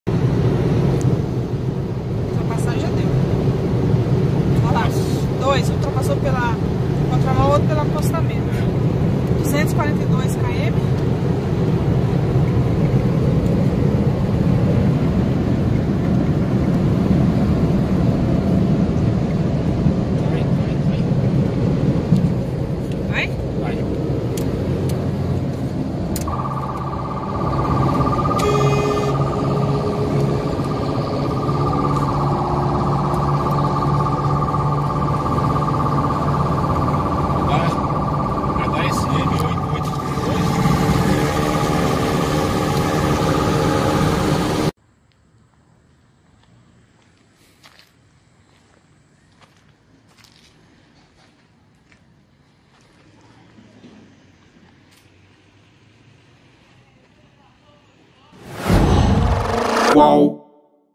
Flagrante: Motociclistas fazem manobra 'superman' e são perseguidos pela polícia
A Polícia Rodoviária Federal (PRF) flagrou ontem três motociclistas dirigindo deitados sobre a moto e com apenas uma das mãos no guidão na altura do km 242 da BR 262, em Ribas do Rio Pardo (MS). De acordo com a PRF, a manobra proibida e extremamente perigosa é conhecida como "superman". Um homem foi detido e os outros dois fugiram..